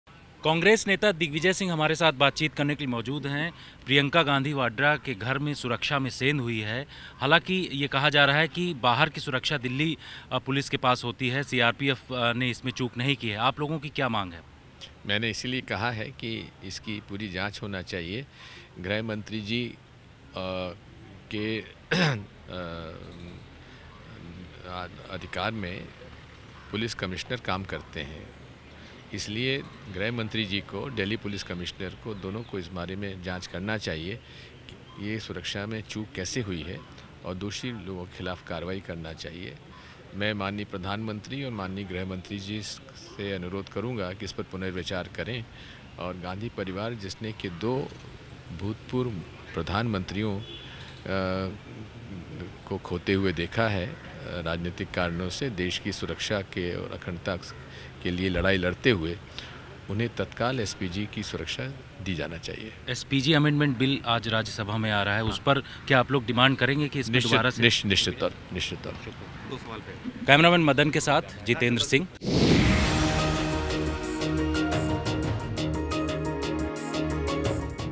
दिग्विजय ने बताया क्यों जरूरी है गांधी परिवार को SPG सुरक्षा ? | MPTAK
दिल्ली में कांग्रेस नेता दिग्विजय सिंह मे एमपी तक से बात की, इस दौरान उन्होंने बताया कि गांधी परिवार को SPG सुरक्षा क्यों&nbsp; जरूरी है, सुनिए उन्होंने क्या कुछ कहा